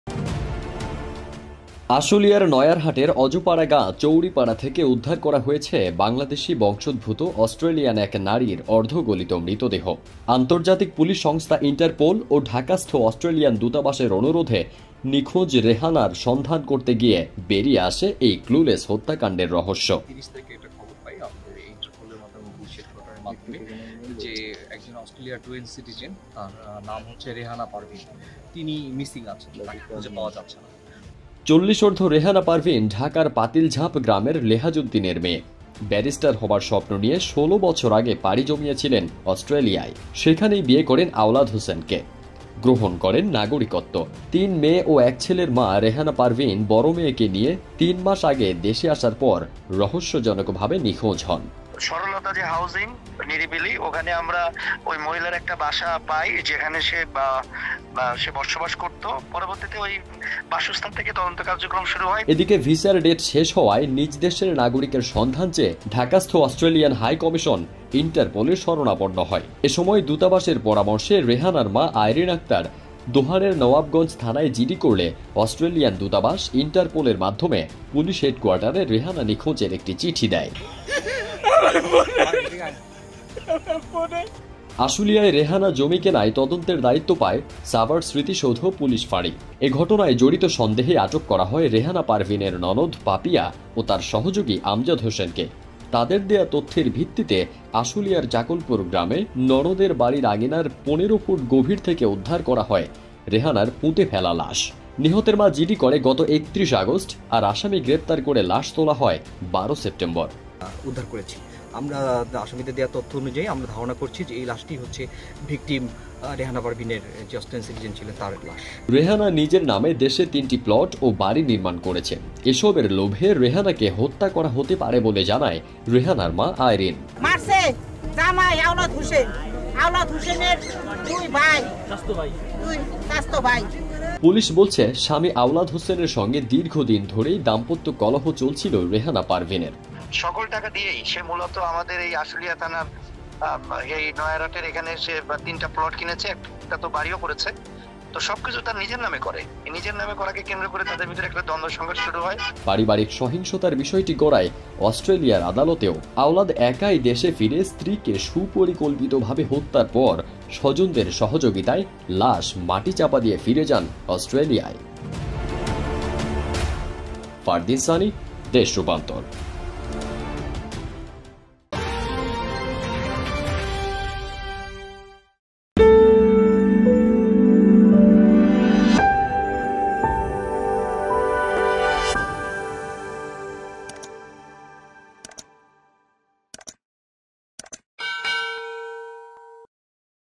আশুলিয়ায় স্ত্রীকে মা-টি চা-পা দিয়ে স্বামীর নাটকীয় প্রস্থান | Ashulia News | Desh Rupantor
News
News